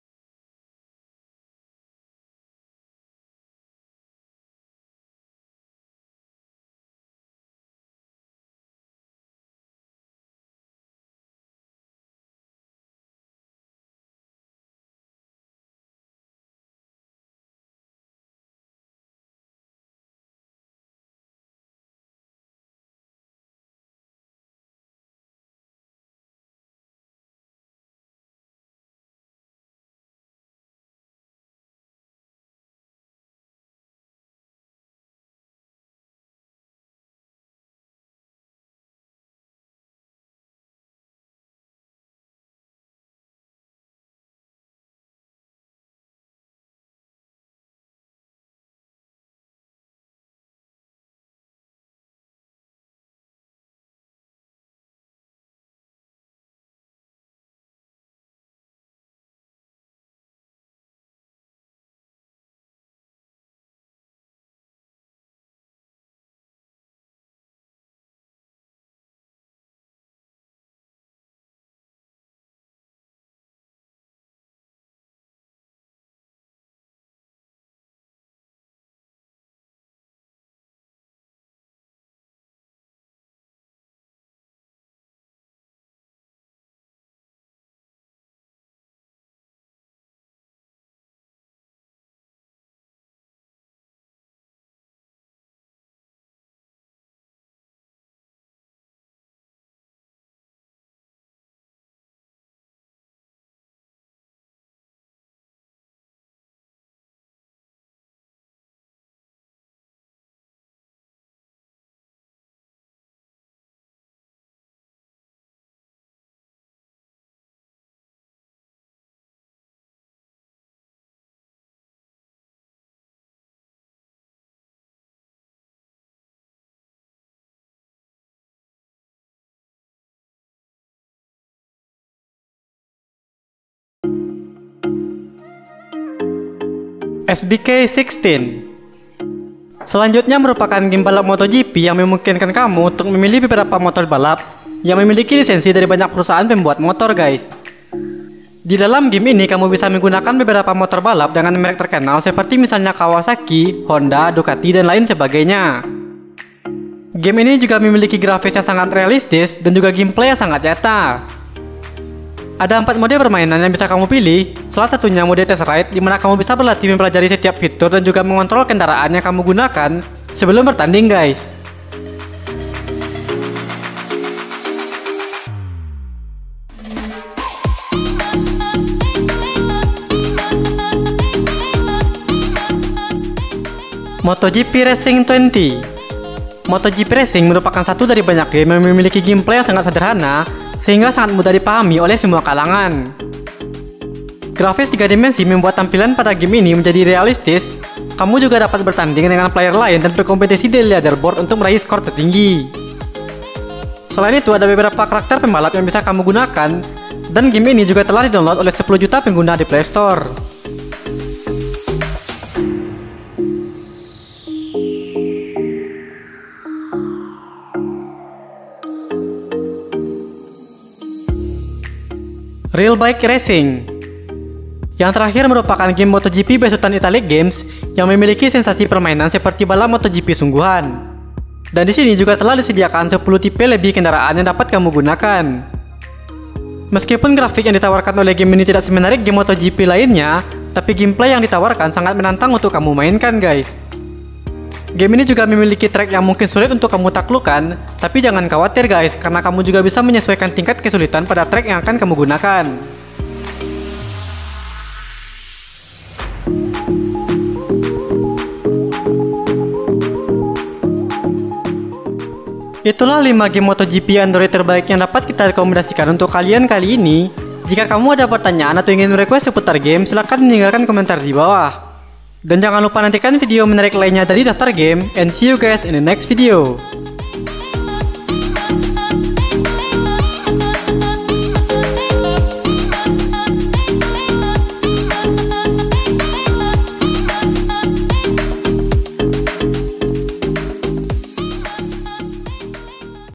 5 Game Bertema MotoGP Terbaik Android 2020
5 Game Bertema MotoGP Terbaik Android 2020

Moto GP merupakan suatu olahraga balap motor yang biasanya di adakan di sebuah cirkuit tertentu. Saat ini motogp menjadi salah satu olahraga yang paling banyak diminati, dan tidak hanya di real life tapi juga di dalam sebuah game.